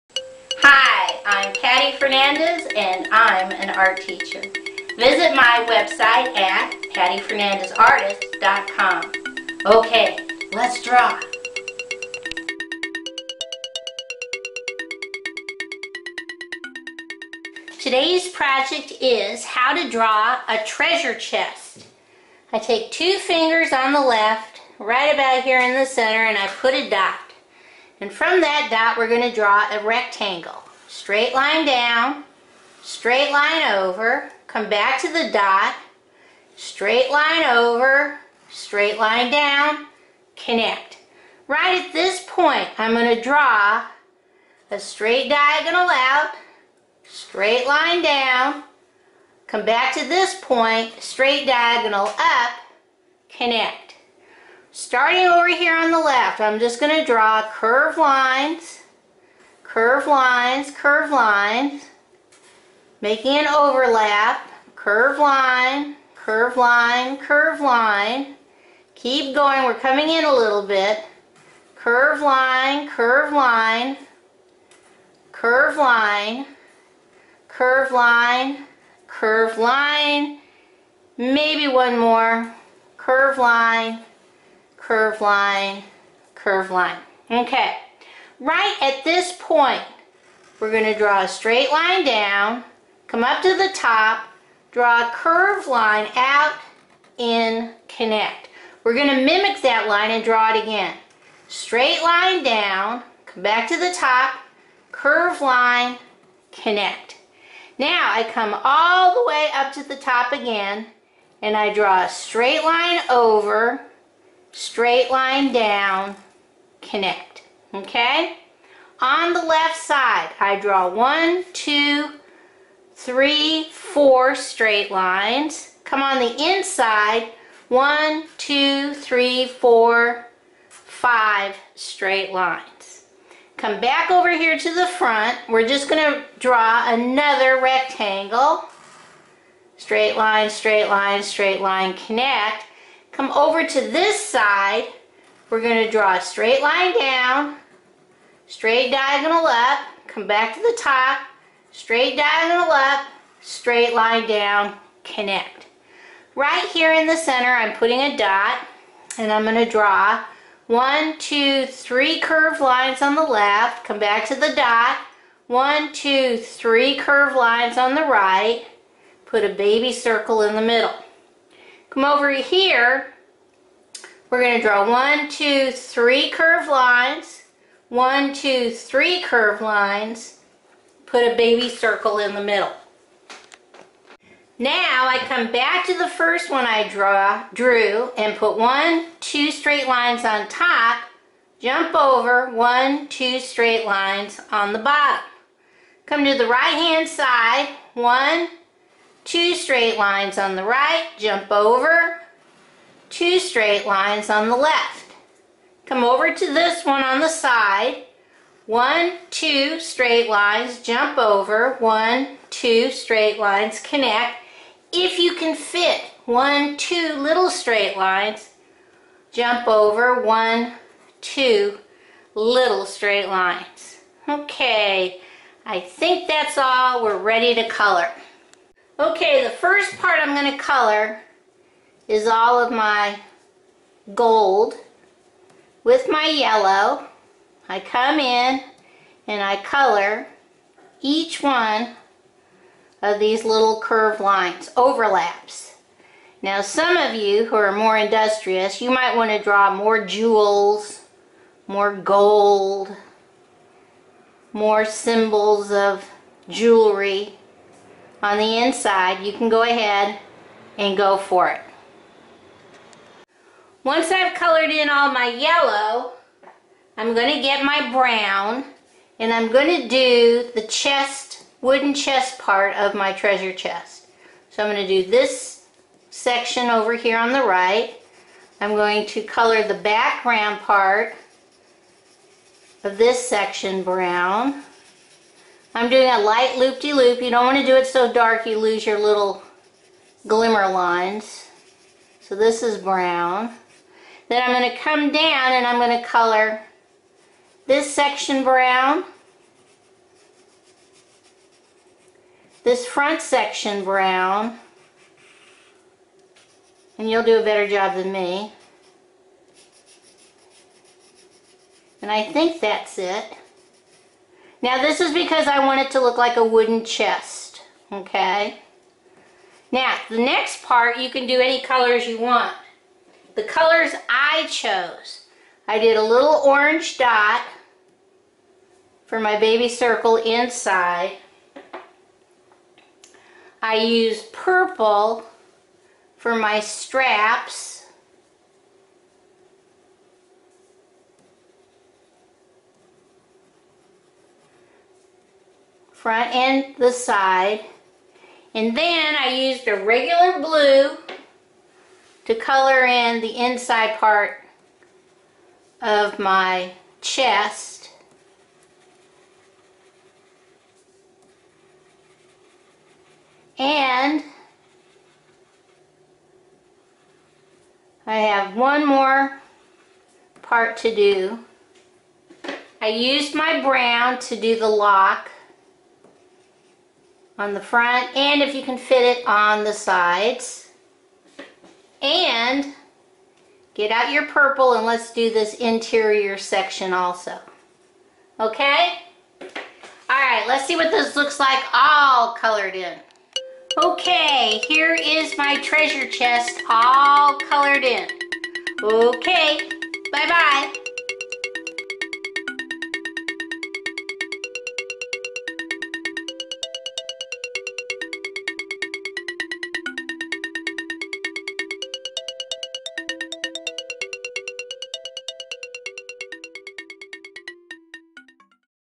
Let's Draw a Treasure Chest!
Easy to follow directions, using right brain drawing techniques, showing how to draw a Treasure Chest.